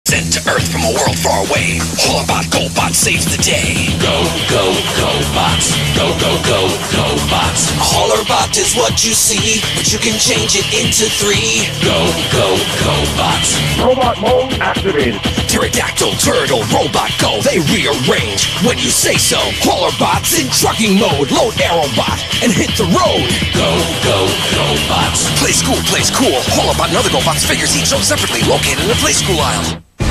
Transformers Go-Bots Hauler-Bot Commercial
Commercial for the Transformers Go-Bots Hauler-Bot figure.
and for early access to the latest pickups consider supporting the archive